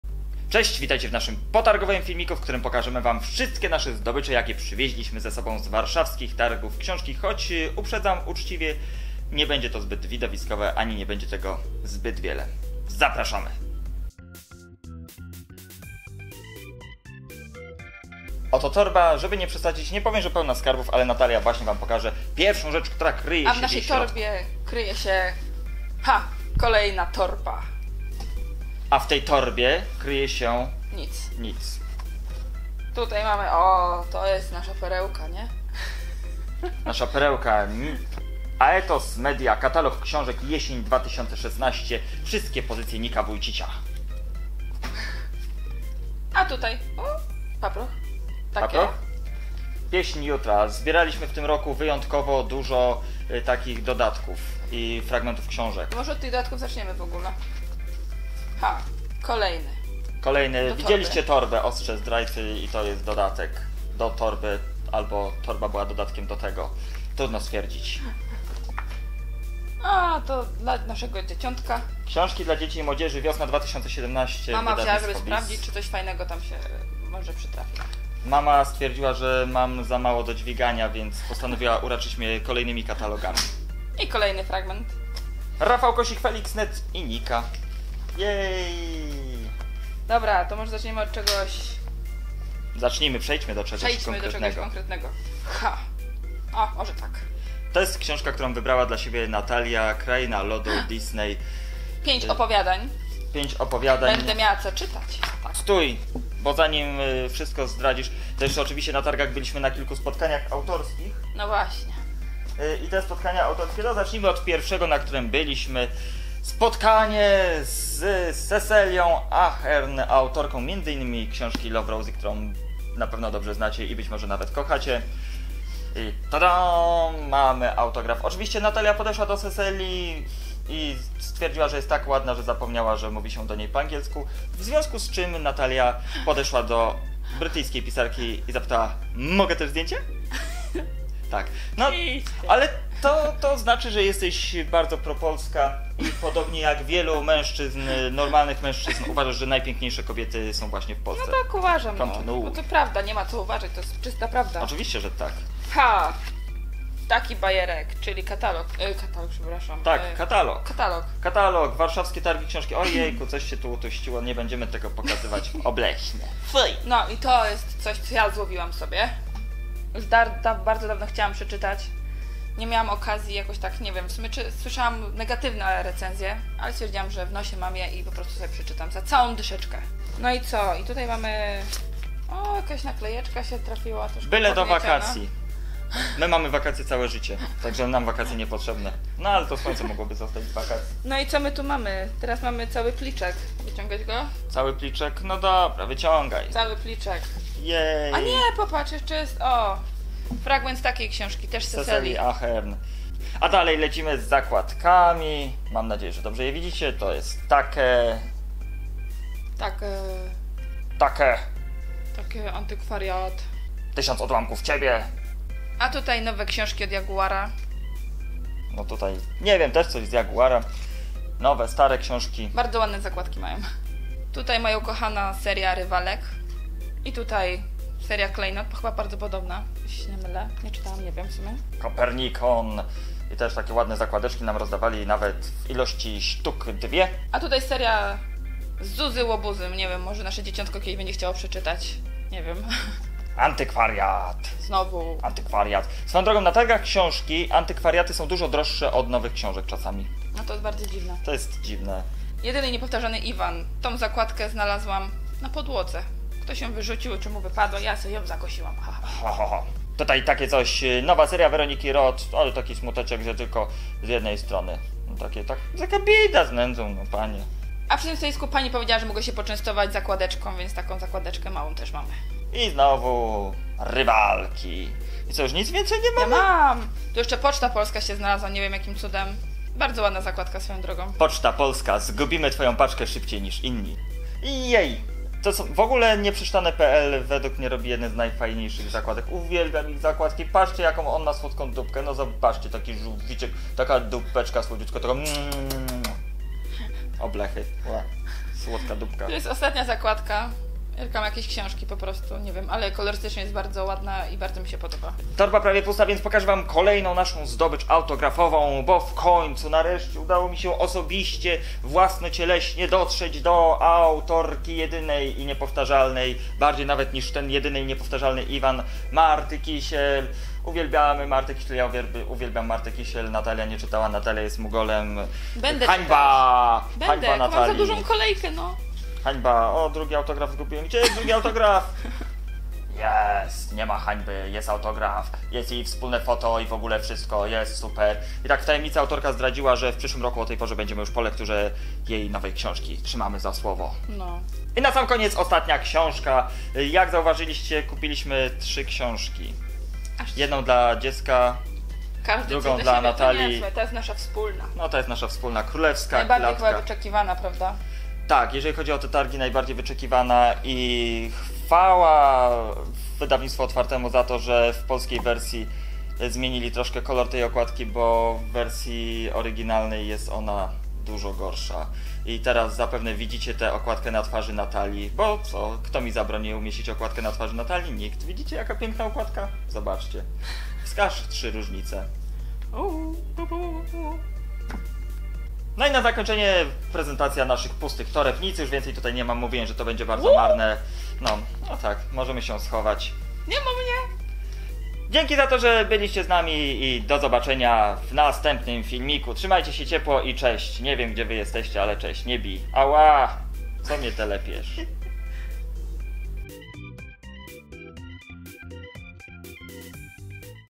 Book Haul z Warszawskich Targów Książki 2017 | Strefa Czytacza
⬇⬇⬇ CZYTAJ WIĘCEJ ⬇⬇⬇

Czas zaprezentować światu nasze targowe zdobycze. :)

Warszawskie Targi Książki to w naszym wypadku pełna chodzenia sobota - w filmie zobaczycie WSZYSTKO to, co udało nam się przywieźć ze stolicy. :)